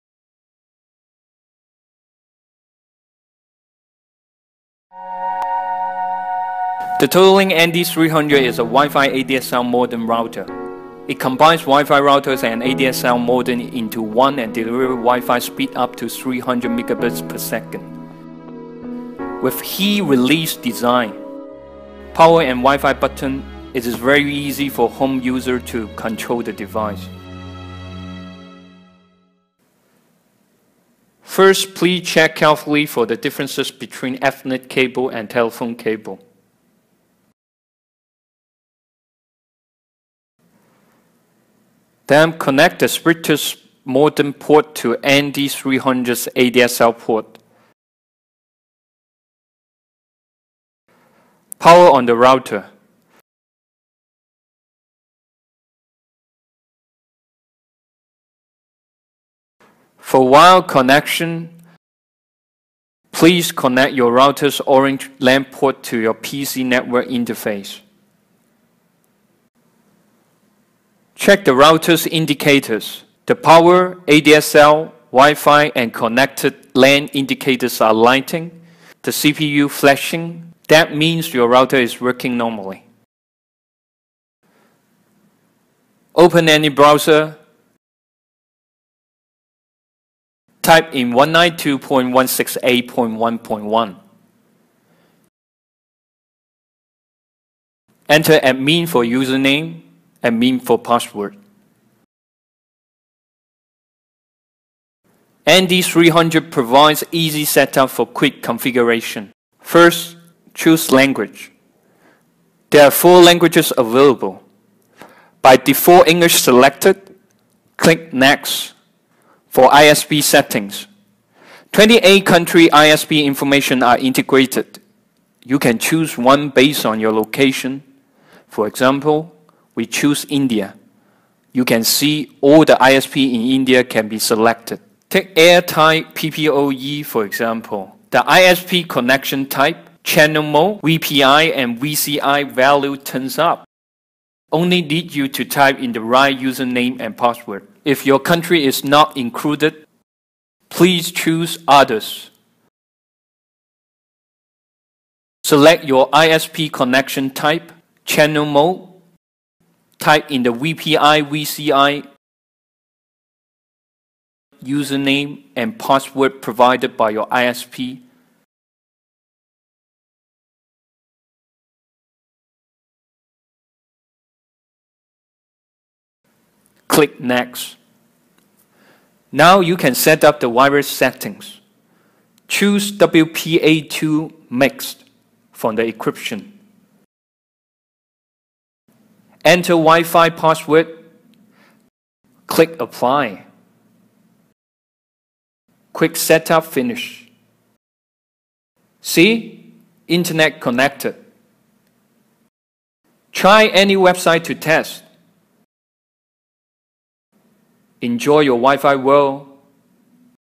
TOTOLINK ND300 - 300Mbps Wireless N ADSL2/2+ Modem Router
As a two-in-one device, ND300 is not hard to install for general home users. It supports multiple lanuages and combines ISP information from about 30 coutries. Only need you to set it up step by step. So easy!